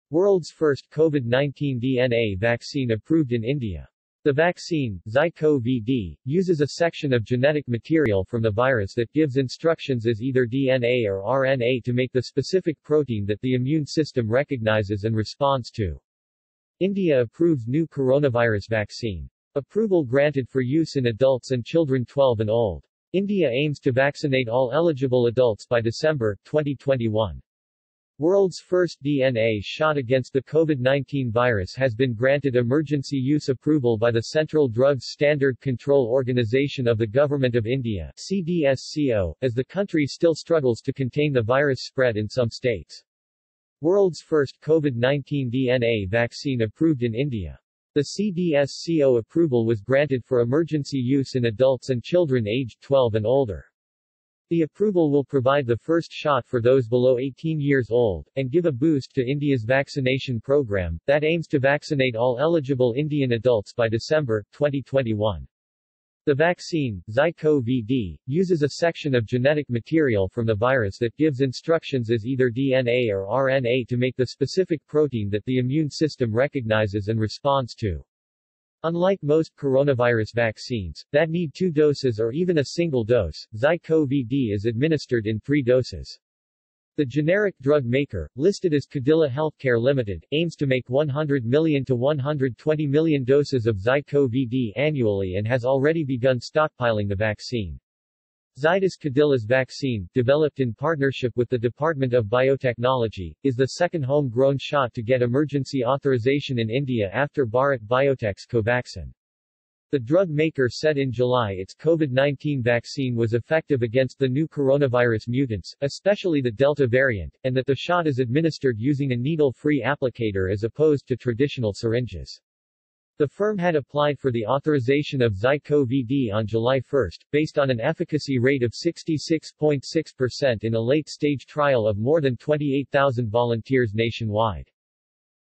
World’s first COVID-19 DNA vaccine approved in India
The vaccine, ZyCoV-D, uses a section of genetic material from the virus that gives instructions as either DNA or RNA to make the specific protein that the immune system recognizes and responds to.